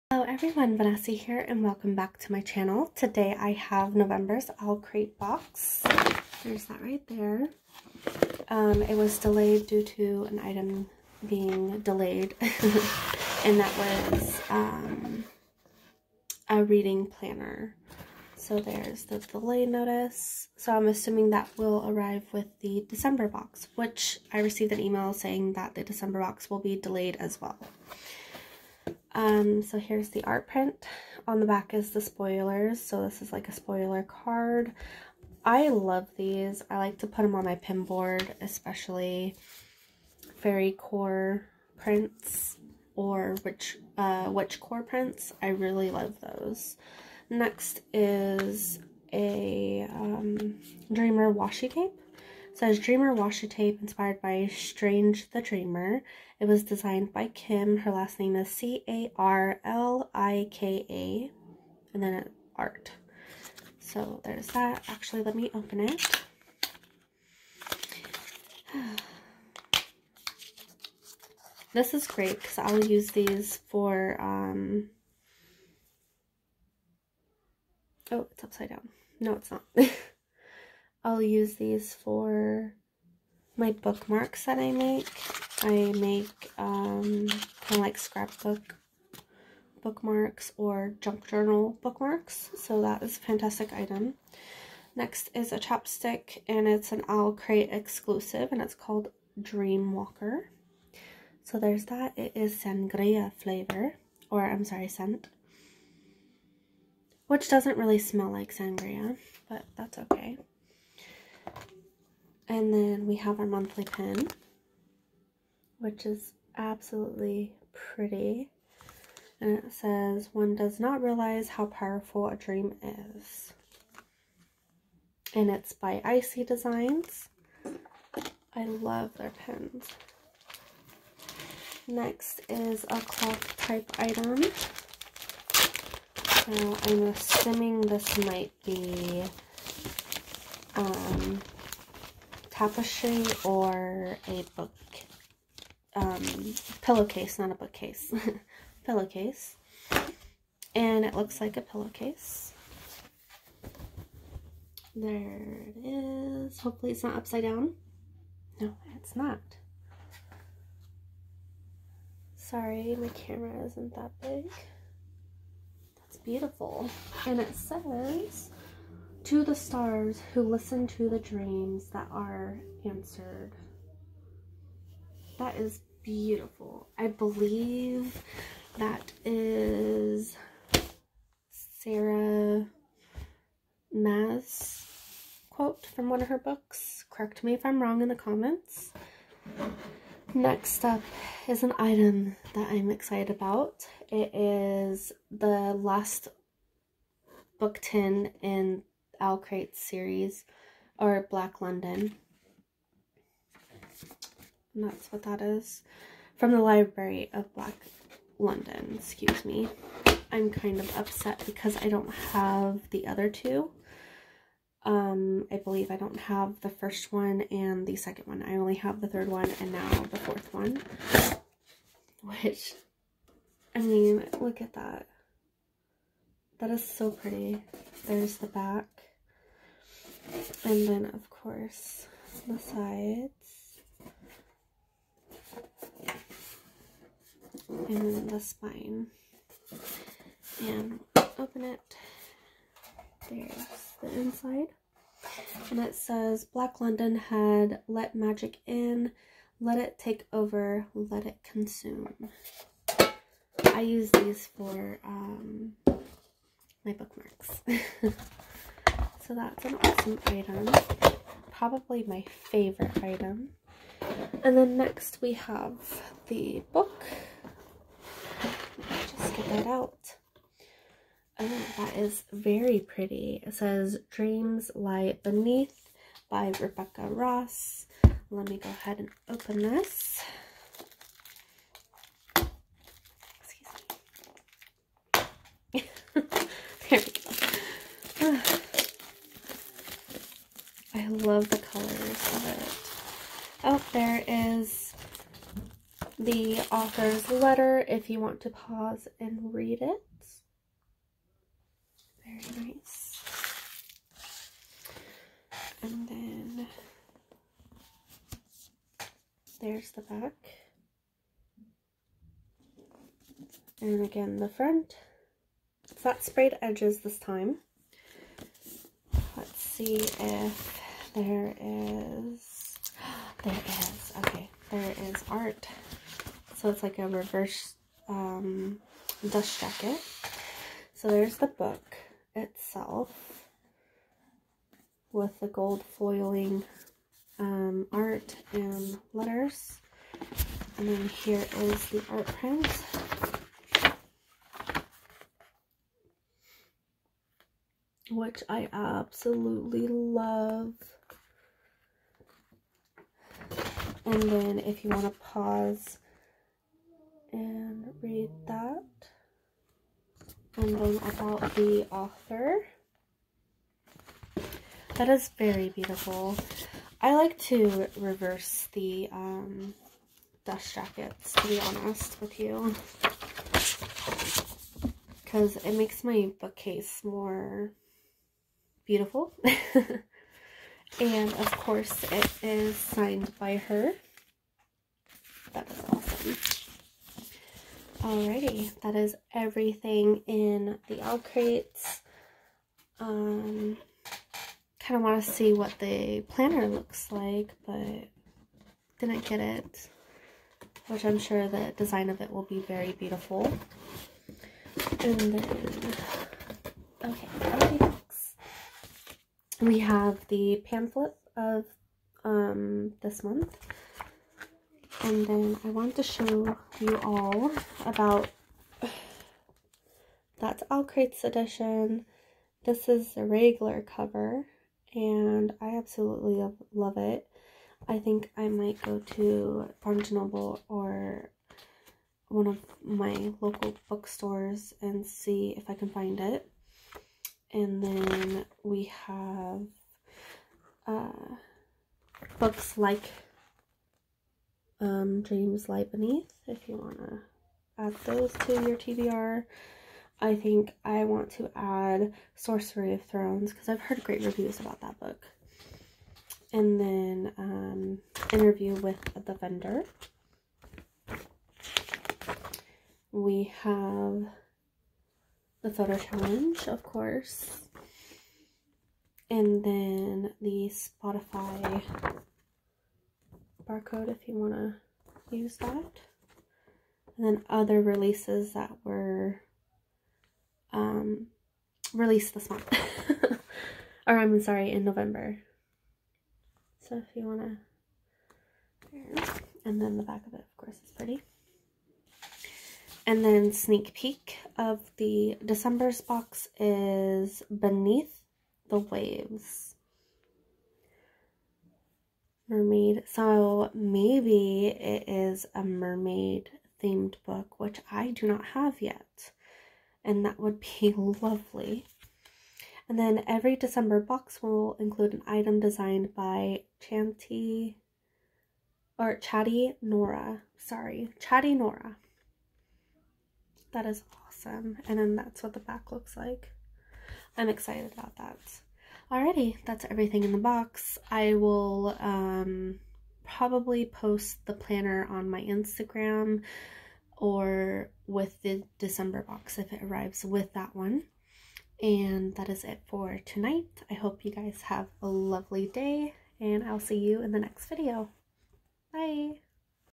November's Owlcrate | Dreaming In The Dark | Unboxing
In the box⬇️

• Final installment of a four part collectible book tin series inspired by A Darker Shade Of Magic designed by Forensics and Flowers
• Sangria flavored lup balm inspired by The Bone Season created by Fiction Bath Co.
• A full size pillowcase inspired by A Court Of Thorns and Roses created by Book Babe Designs
• Washi Tape inspired by Strange the Dreamer created by Kim Carlika Art
• This month's enamel pin inspired by Dreams Lie Beneath created by Icey Designs
• November's book is, Dreams Lie Beneath by Rebecca Ross with a redesigned exclusive cover designed by Chatty Nora with a reversible dust jacket with artwork created by Dri Gomez on the other side.
*Missing item is @paperbackbones reading planner*

Shout out to @Meandmy Tea and @ForTheLoveOfBooks your videos inspired me to get Owlcrate Subscription Box 😁🧡

**December's theme is Beneath the Waves**